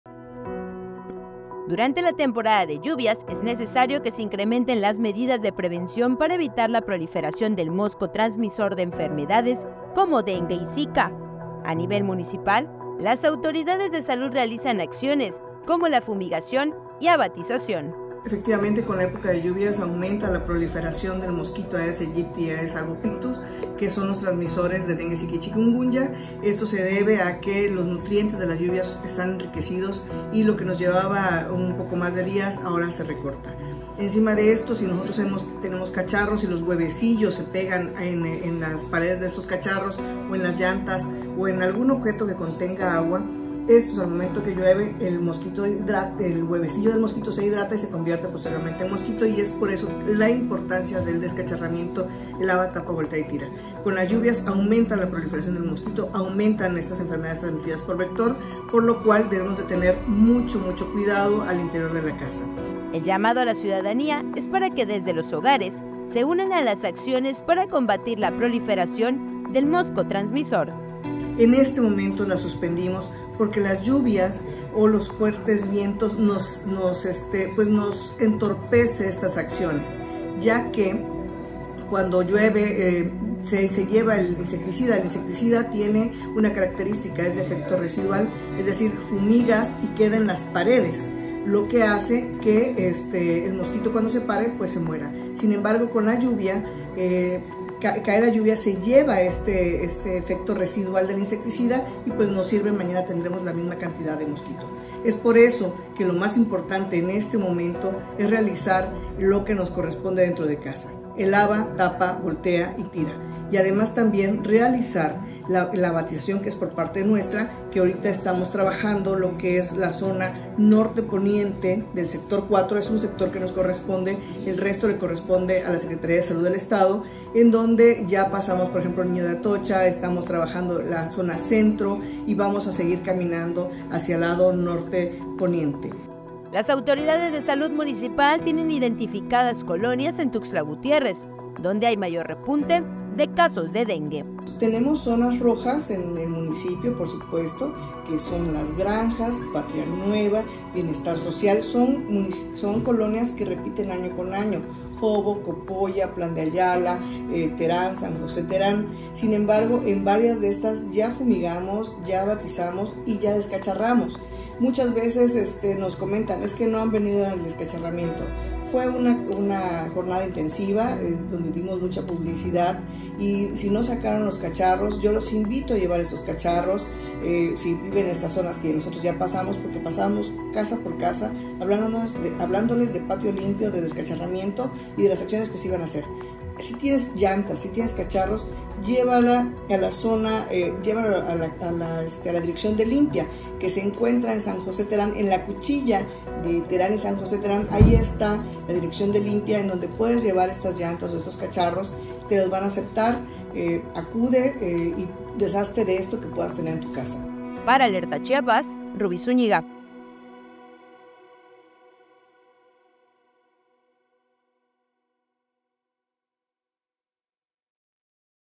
Fumigación y abatizacion contra el mosco transmisor del dengue
Han identificado colonias en Tuxtla Gutiérrez donde hay repunte de casos de dengue.